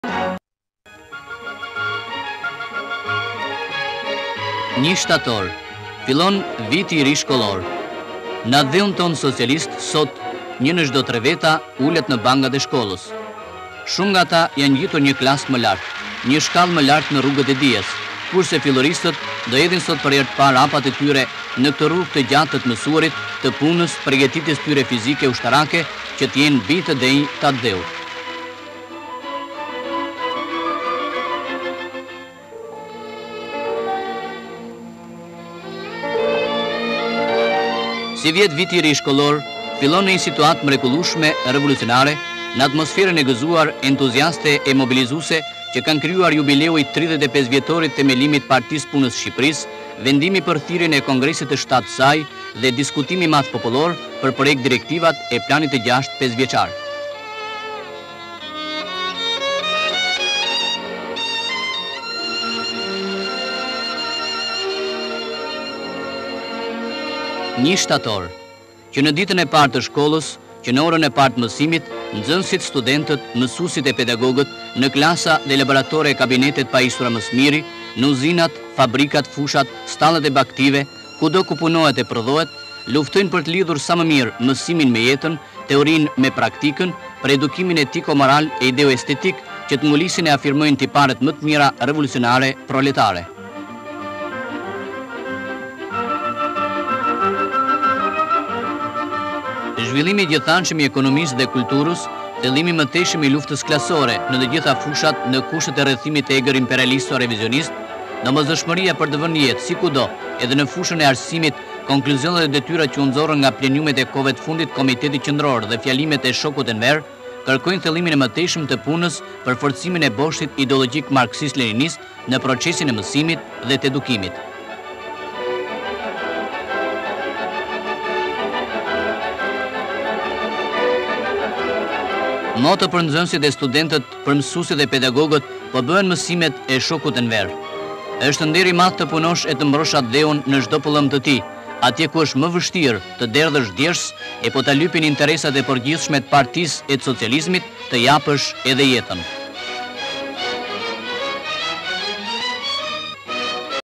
Fillimi i vitit te ri shkollor. Nxenesit me tufa me lule qendrojne para shkolles.
Fillimi i vitit te ri shkollor. Nxenesit me tufa me lule qendrojne para shkolles. Hyjne ne klase te pare. Mesuesja duke u folur. Pamje e shkolles se mesme bujqesore ne Kavaje. Nxenesit duke mesuar ne klase dhe ne praktike. Mesuesi duke ju shpjeguar, nxenesit duke bere praktiken ne vjeljen e rrushit. Nxenesit te veshur me rroba ushtarake.